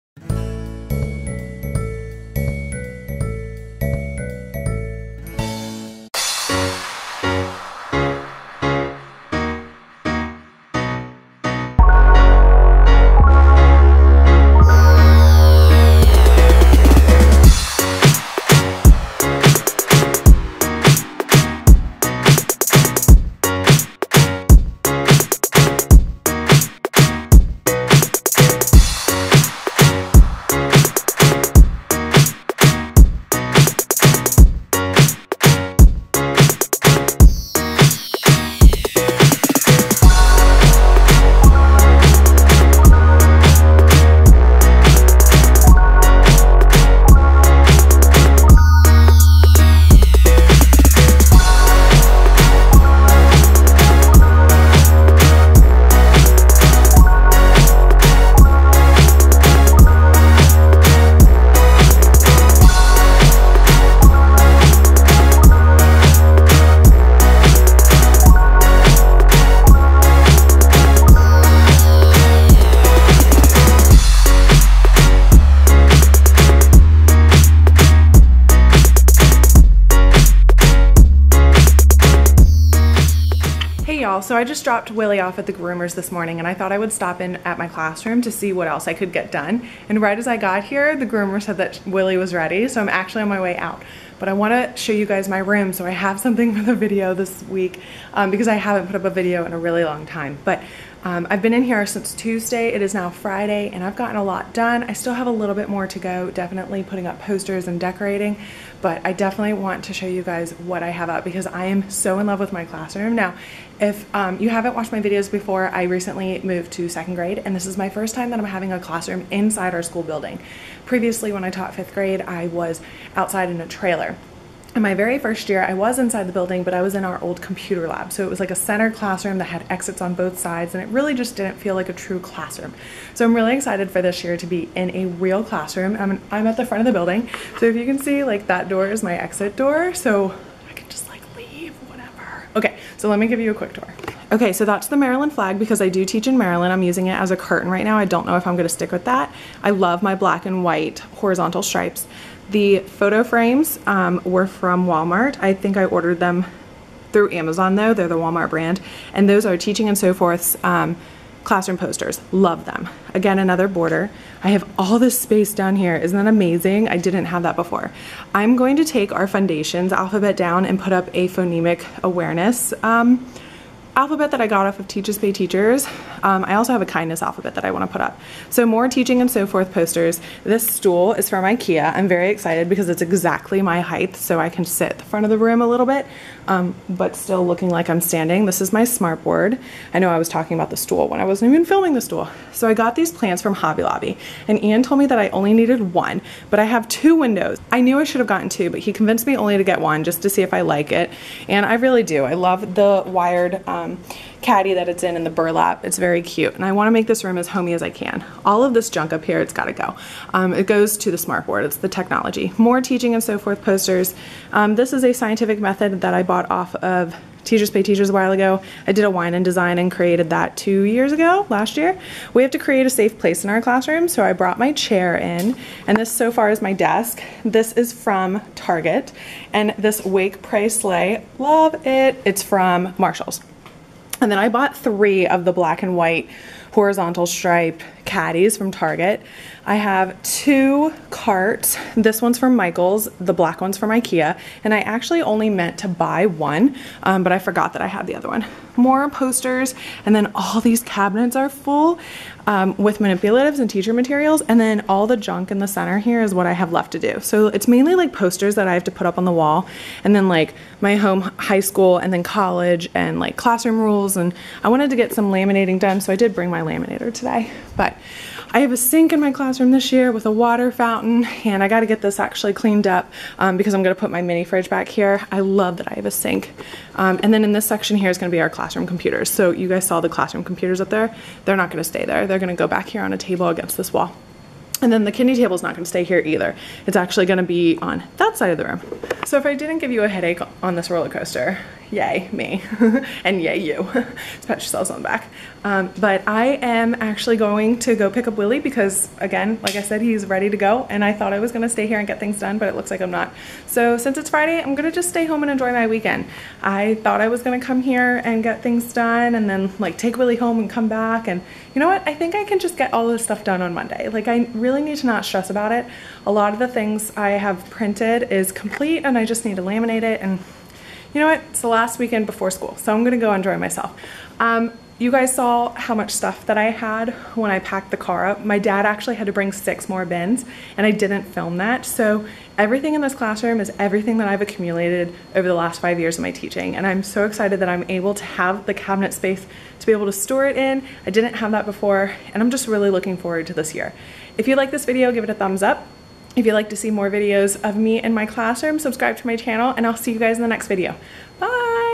GETTING MY CLASSROOM READY | A TEACHER'S LIFE FOR ME EP24
After watching a few teacher vlogs, I became inspired to create my own.

Plants from Hobby Lobby

Hanging File Folder from Target

Wake. Pray. Slay. from Marshalls

Fabric for boards from Walmart

Maryland Flag from auction

Black boarder from Staples